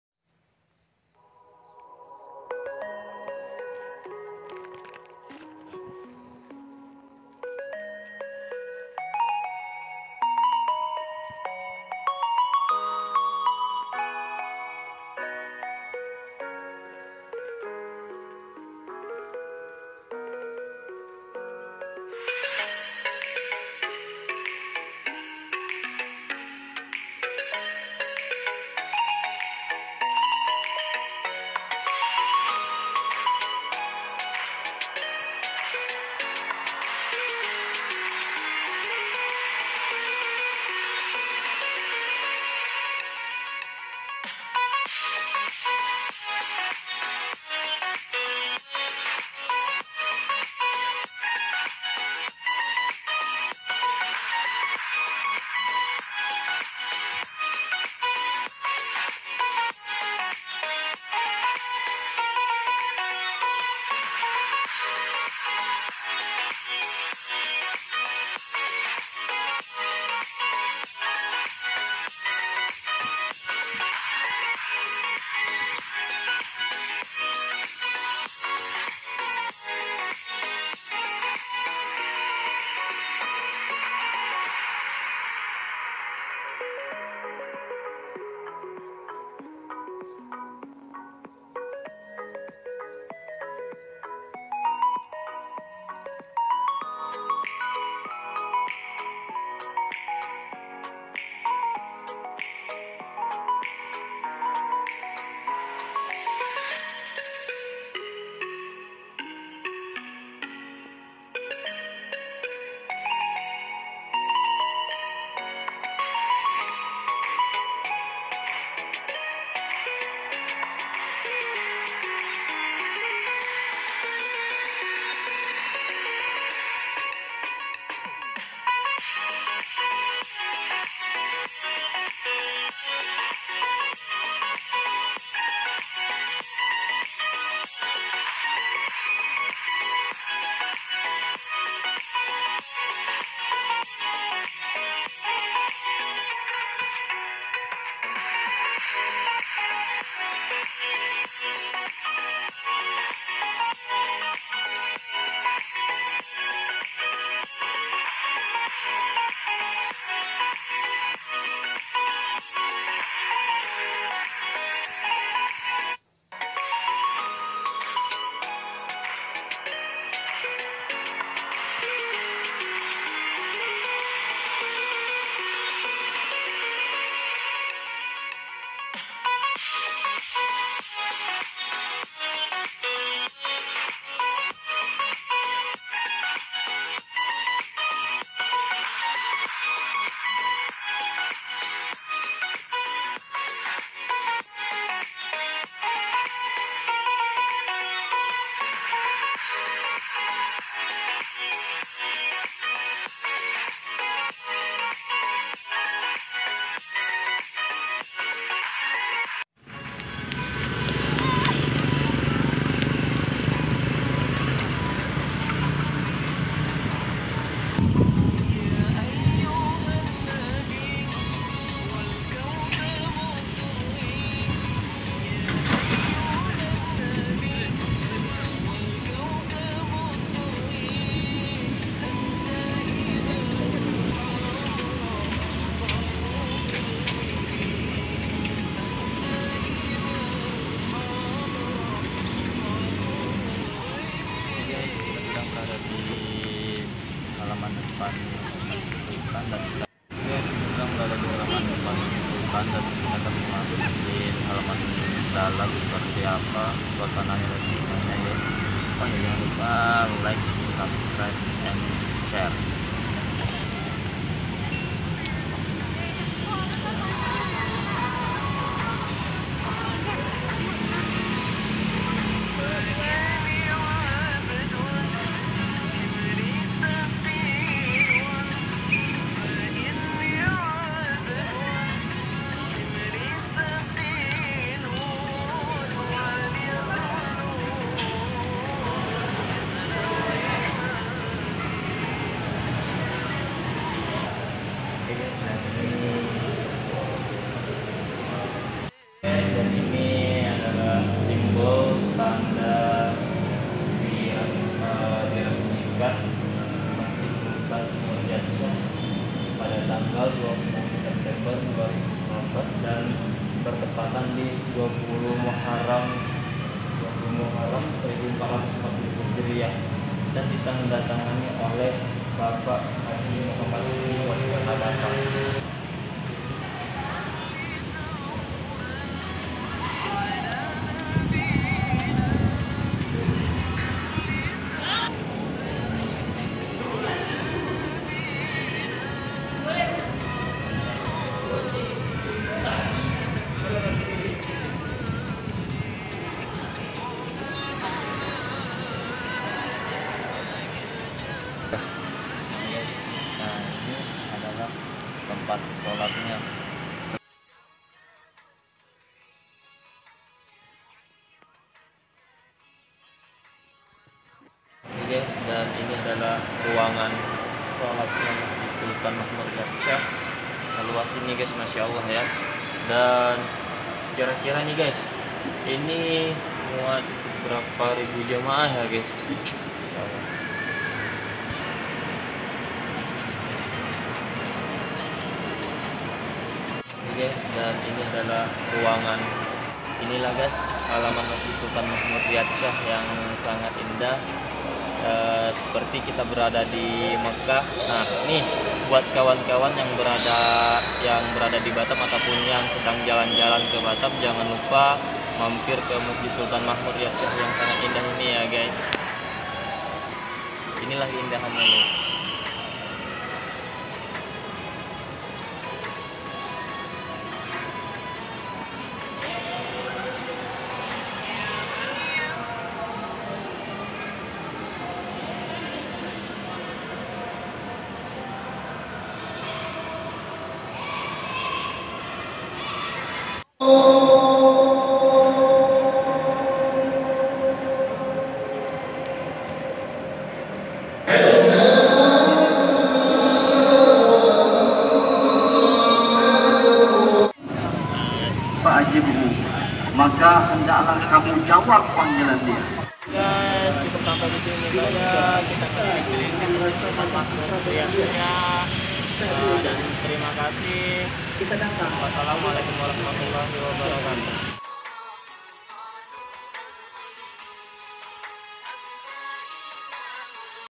MESJID SULTAN MAHMUD RIAYAT SYAH BATAM
Masjid Sultan Mahmud Riayat Syah akan menjadi salah satu ikon Kota Batam dan bakal menjadi daya tarik bagi wisata religi